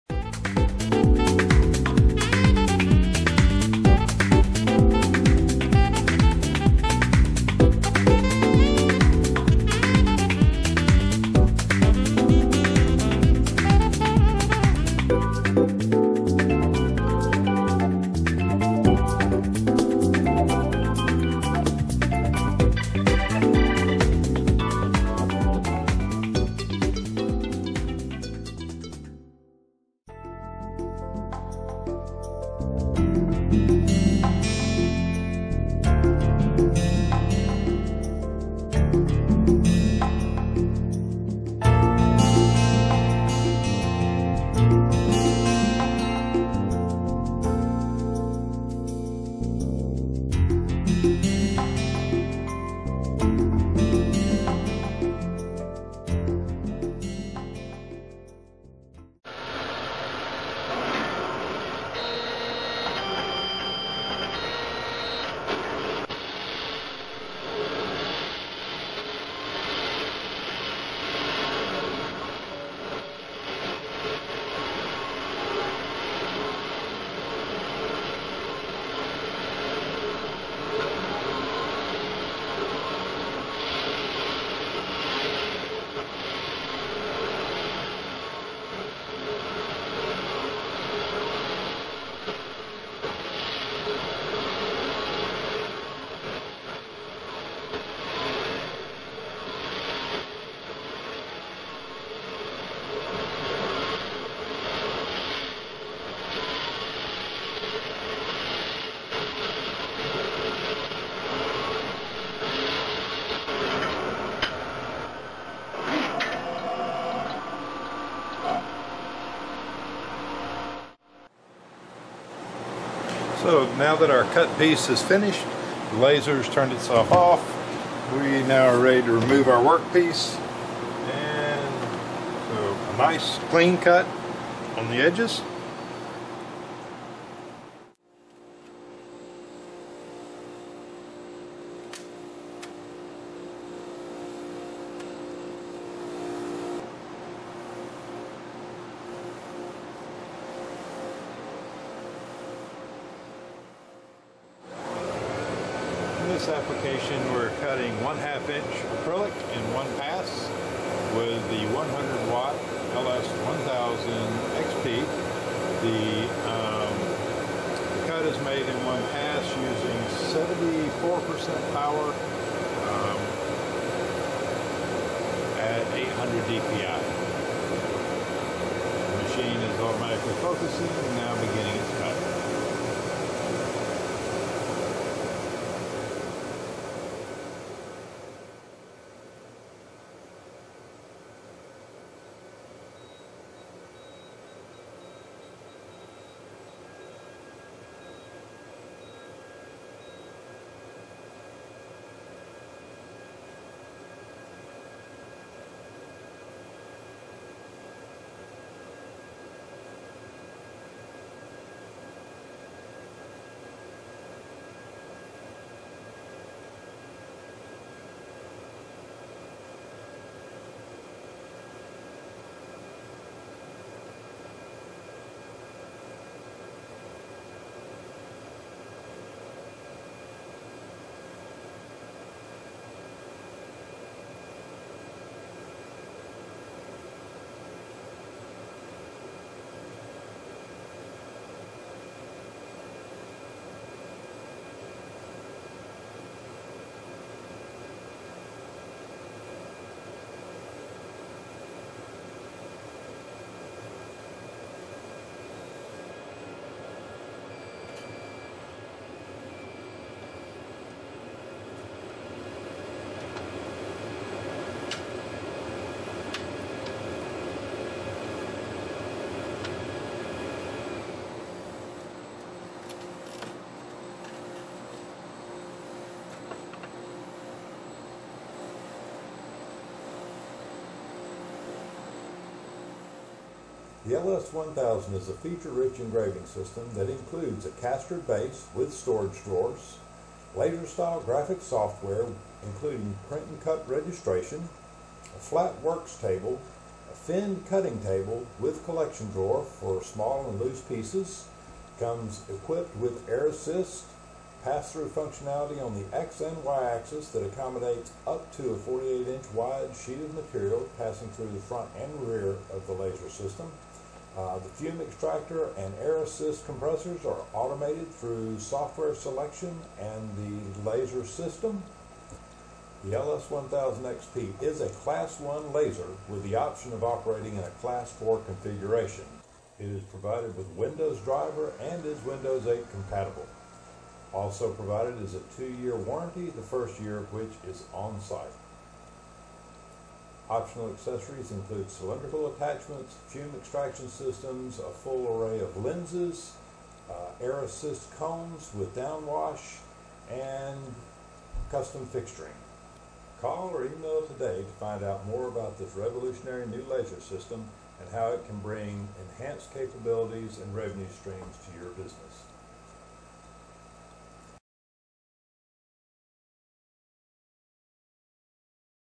LS1000XP CO2 Laser Overview
The LS1000XP is a feature rich CO2 laser that has something for everyone.  Available power ranges from 40 to 150 watts and a marking area of 24x48", the LS1000XP meets all challenges.  Standard features include print & cut registration, full pass through of sheet up to 48" wide on the y axis and 24 " wide on the x axis. Recognition shovels, oversized panels, memorial walls and totem poles are no longer a challenge.